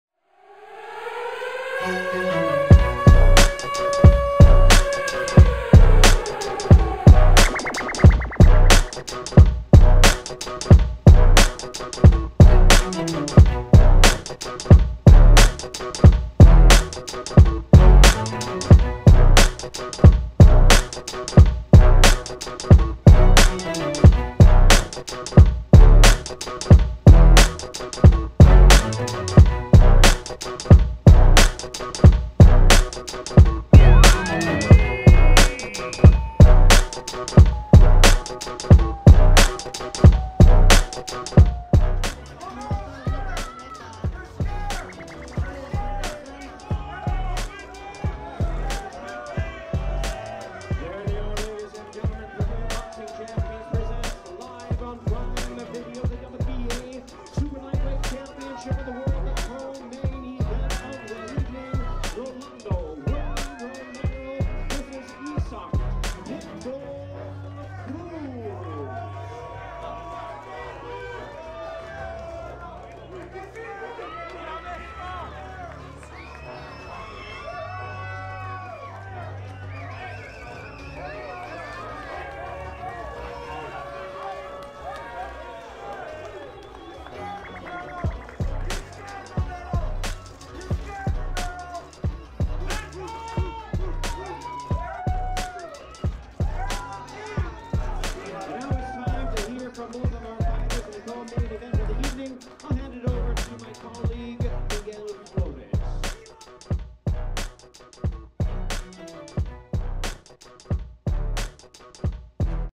Gervonta Davis REACTS to Isaac Cruz KNOCKING OUT Rolly Romero in 8
Jr. middleweight champion Tim Tszyu takes on top contender Sebastian Fundora and jr. welterweight champion Rolly Romero faces Isaac Cruz in the inaugural PBC on Prime Video card on March 31 at T-Mobile Arena in Las Vegas, Nevada. You don't want to miss what goes down in the events leading up to the fight. Check it out!